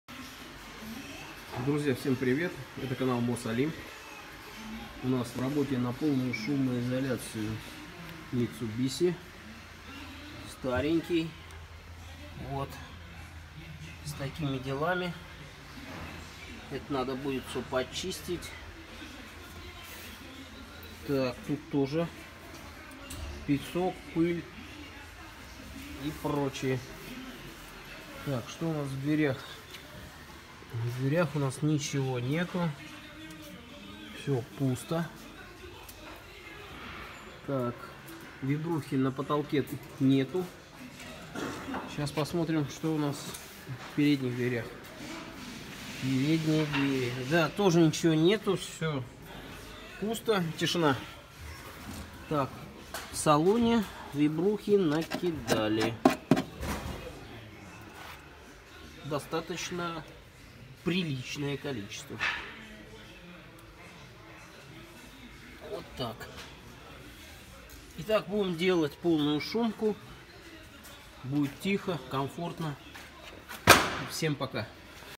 Старенький Лансер 9, странно узнать, что на самом-то деле у него есть немного штатной шумоизоляции
Современный темп жизни в больших городах порождает и много шума тоже, а обилие шума порождает желание автовладельцев иметь машину, в которой будет тише некуда. И мы вполне можем удовлетворить это желание, да и не только мы, а, например, премиальные автомобили, но ценник у них достаточно высоковат, а отдельно сделанная полная шумоизоляция обойдется всего лишь в цену нескольких техосмотров у официального дилера. За этим к нам и заглянул этот лансер, и парни уже приступили к полному разбору машины, в ходе которого как раз и можно внимательно посмотреть на то, что стало причиной попадания машины к нам в сервис - небольшое количества заводской шумоизоляции, да еще и посредственного качества.